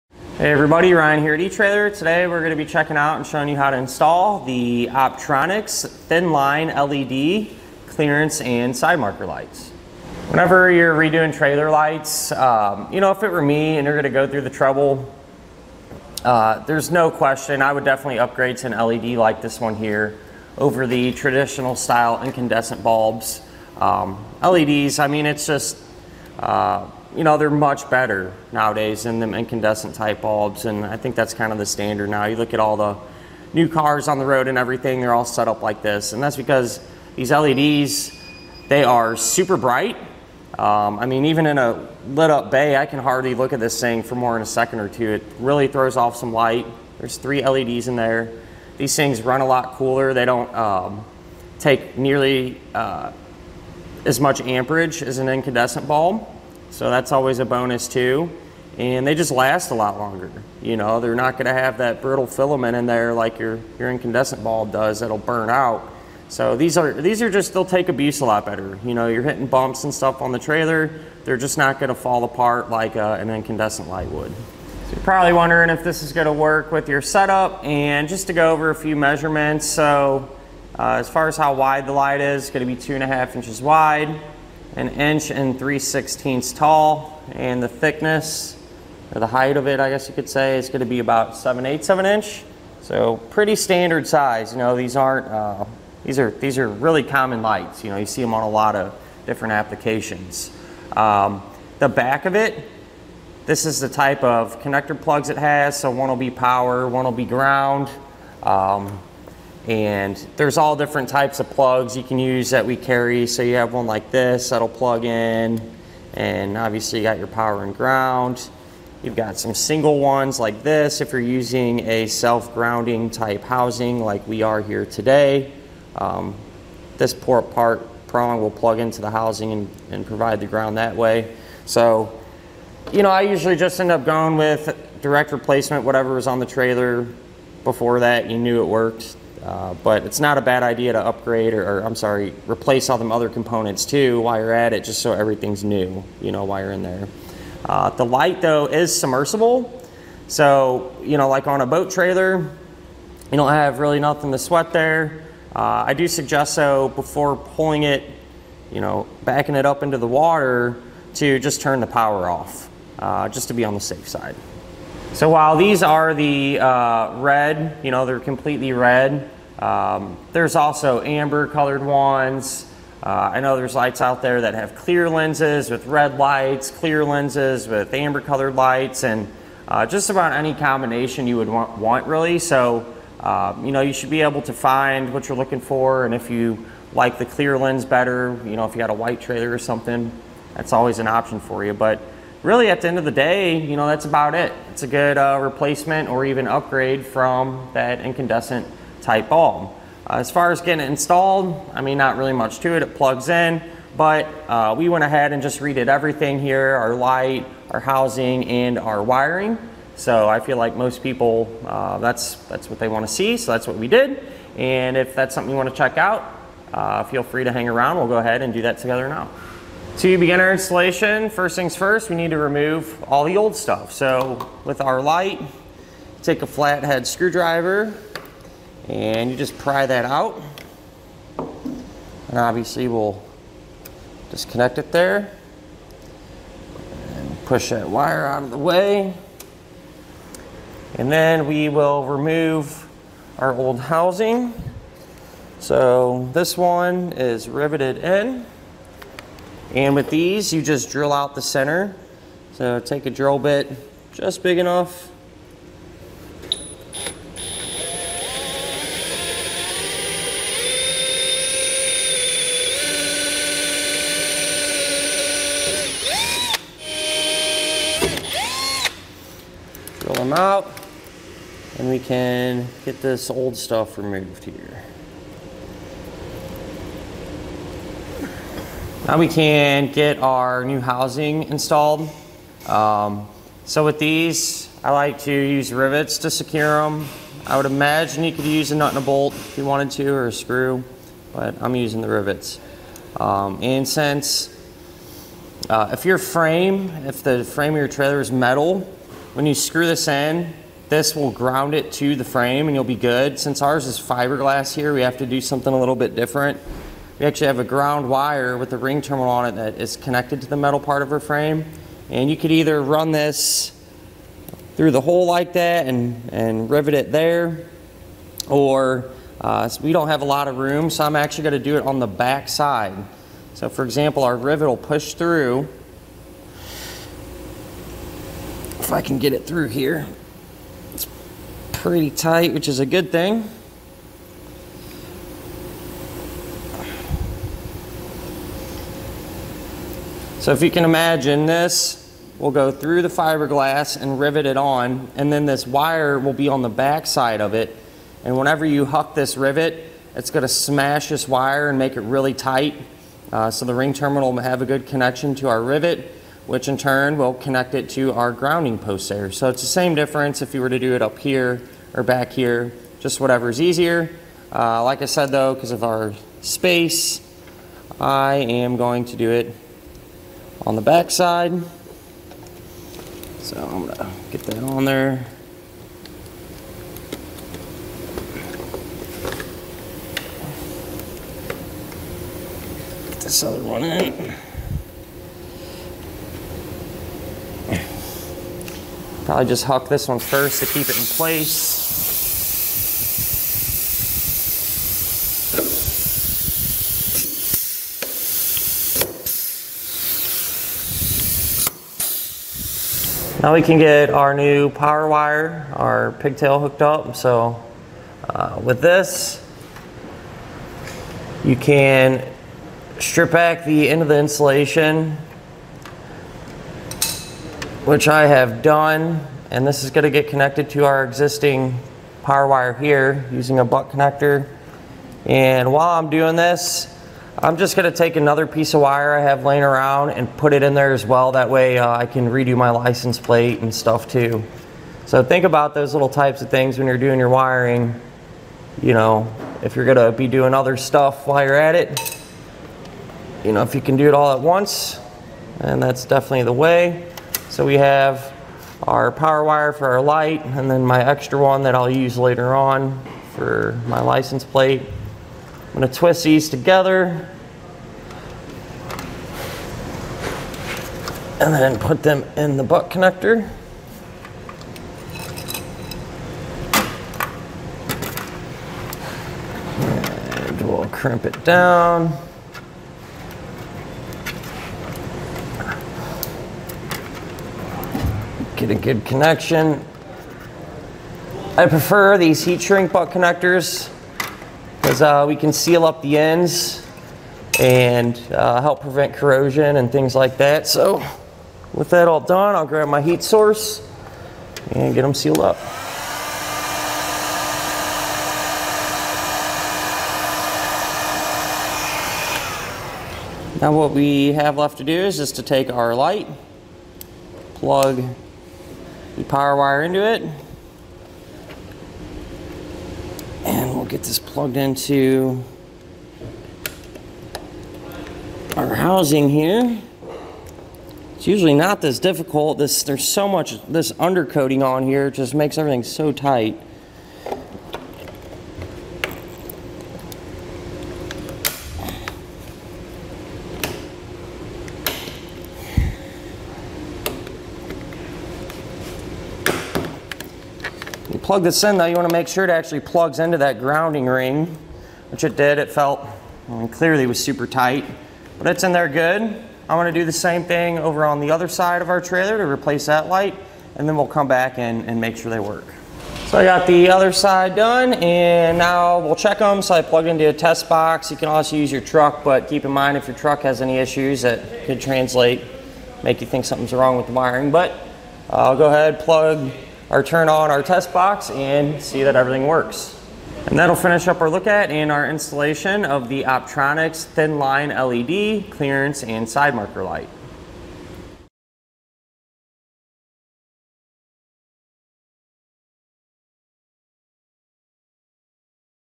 etrailer | Detailed Breakdown of the Thinline LED Clearance and Side Marker Light
Hey everybody, Ryan here at etrailer. Today, we're gonna be checking out and showing you how to install the Optronics Thinline LED Clearance and Side Marker Lights. Whenever you're redoing trailer lights, you know, if it we're me, and you're gonna go through the trouble there's no question, I would definitely upgrade to an LED like this one here, over the traditional style incandescent bulbs. LEDs, I mean, it's just, you know, they're much better nowadays than incandescent type bulbs, and I think that's kind of the standard now. You look at all the new cars on the road and everything, they're all set up like this and that's because these LEDs, they are super bright. I mean, even in a lit up bay I can hardly look at this thing for more than a second or two, it really throws off some light.

There's three LEDs in there. These things run a lot cooler, they don't take nearly as much amperage as an incandescent bulb, so that's always a bonus too. And they just last a lot longer. They're not gonna have that brittle filament in there like your incandescent bulb does that'll burn out. So these are just, they'll take abuse a lot better.

You're hitting bumps and stuff on the trailer, they're just not gonna fall apart like an incandescent light would. So probably wondering if this is gonna work with your setup, and just to go over a few measurements. So as far as how wide the light is it's gonna be 2.5 inches wide, an inch and 3/16 tall, and the thickness or the height of it, I guess you could say, is gonna be about 7/8 of an inch. So pretty standard size. These are really common lights, you know, you see 'em on a lot of different applications.

The back of it, this is the type of connector plugs it has, so one will be power, one will be ground. And there's all different types of plugs you can use that we carry. So you have one like this that'll plug in, and obviously you got your power and ground. You've got some single ones like this, if you're using a self grounding type housing like we are here today. This prong will plug into the housing and provide the ground that way.

So I usually just end up going with direct replacement whatever was on the trailer before that, you knew it worked. But it's not a bad idea to upgrade or, I'm sorry, replace all them other components too while you're at it, just so everything's new, you know, while you're in there. The light though is submersible, so like on a boat trailer you don't have really nothing to sweat there. I do suggest though before pulling it, backing it up into the water, to just turn the power off just to be on the safe side. So while these are the red, they're completely red, there's also amber colored ones. I know there's lights out there that have clear lenses with red lights, clear lenses with amber colored lights, and just about any combination you would want want really. So, you know, you should be able to find what you're looking for. And if you like the clear lens better, you know, if you've got a white trailer or something, that's always an option for you. But really at the end of the day, that's about it. It's a good replacement or even upgrade from that incandescent type bulb. As far as getting it installed, I mean, not really much to it, it plugs in. But we went ahead and just redid everything here, our light, our housing, and our wiring. So I feel like most people that's what they wanna see, so that's what we did. And if that's something you wanna check out feel free to hang around, we'll go ahead and do that together now. So to begin our installation, first things first, we need to remove all the old stuff. So with our light, take a flat head screwdriver and you just pry that out. And obviously we'll disconnect it there, and push that wire out of the way. And then we will remove our old housing. So this one is riveted in, and with these, you just drill out the center. So take a drill bit, just big enough. Drill them out, and we can get this old stuff removed here. Now we can get our new housing installed. So with these, I like to use rivets to secure 'em. I would imagine you could use a nut and a bolt if you wanted to, or a screw, but I'm using the rivets. And since if your frame of your trailer is metal, when you screw this in, this will ground it to the frame and you'll be good. Since ours is fiberglass here we have to do something a little bit different. We actually have a ground wire with the ring terminal on it that is connected to the metal part of our frame, and you could